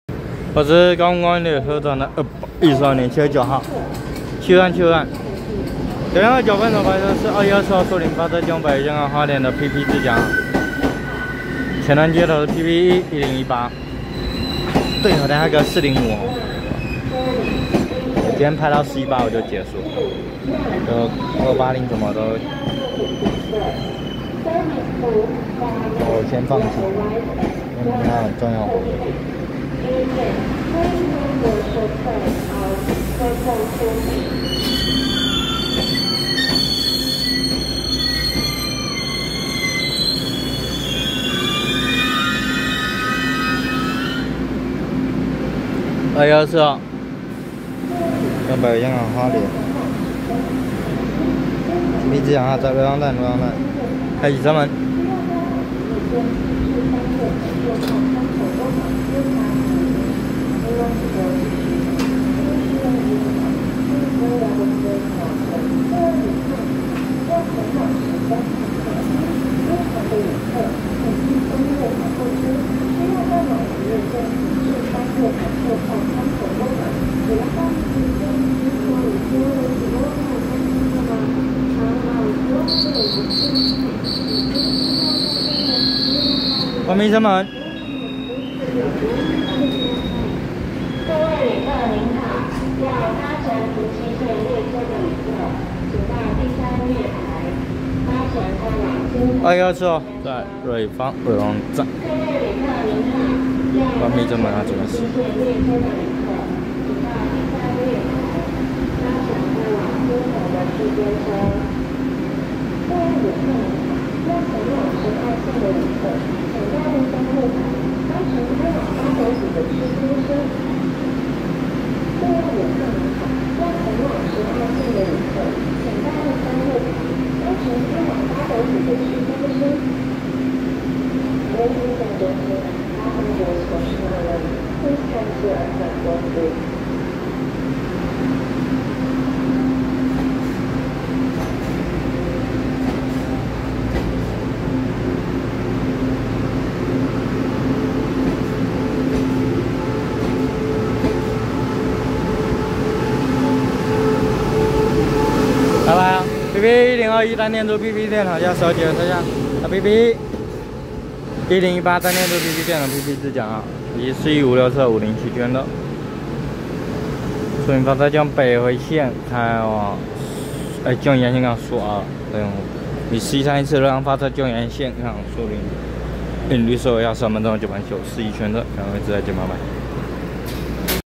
212次樹林發車開往花蓮的PPE1021單電豬PP電頭+十二節PP車廂+PPE1018單電豬PP電頭PP自強號，4156次瑞芳發車經由宜蘭線開往蘇澳的單組EMU500型四輛一編組EMU507區間車
112/7/9瑞芳站9:29分拍攝212次樹林發車經由北迴線開往花蓮的PPE1021單電豬PP電頭+十二節PP車廂+PPE1018單電豬PP電頭PP自強號，4156次瑞芳發車經由宜蘭線開往蘇澳的單組EMU500型四輛一編組EMU507區間車